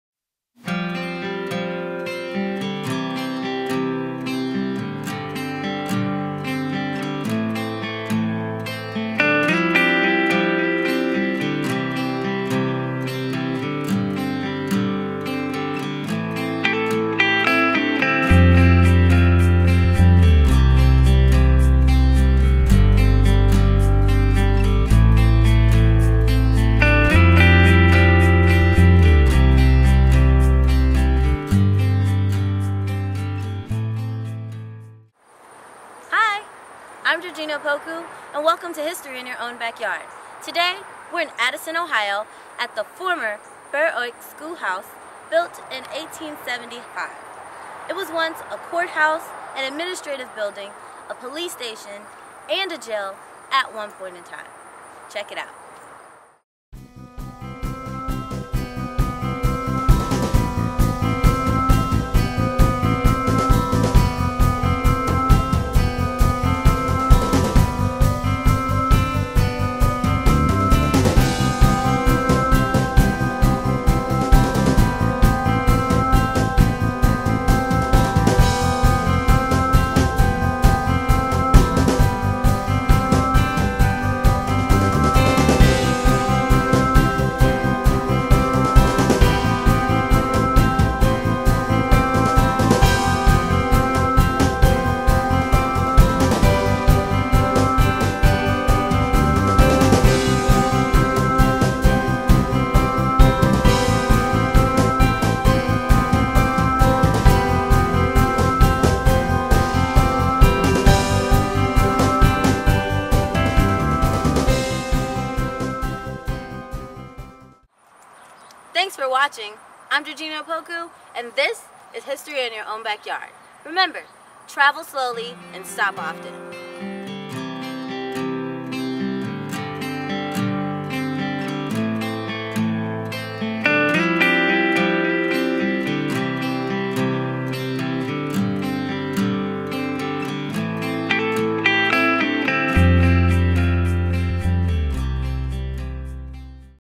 How Many Schools Have Been Built In Addyston, Ohio?
The  Burr  Oak  School  House  was  preceeded  by  two  smaller  one  room  school  houses.  One  school  was  built  in  1850  and  located  at  Cleves  Pike  and  South  Road.  The  other  school  was  built  in  1869  and  located  near  Shady  Lane  and  River  Road.  The  population  of  nearby  North  Bend  began  to  expand  causing  a  greater  need  for  a  larger  swchool  house  for  the  area  children.

In  February  1875  an  acre  of  ground  was  purchased  from  John  Cleves  Short  to  build  a  new  school  called  "Burr  Oak".  By  1891  the  Burr  Oak  School  was  too  small  to  handle  the  number  of  children  flowing  into  the  system  from  workers  at  the  new  Addyston  Pipe  &  Steel  Company.

Soon  the  Red  Brick  School  was  constructed  and  used  up  until  it  also  became  outdated  and  too  small.

In  1937  a  new  school  was  built  called  the  Meredith  Hitchens  Elementary  School  and  was  used  until  it  was  demolished  in  2012.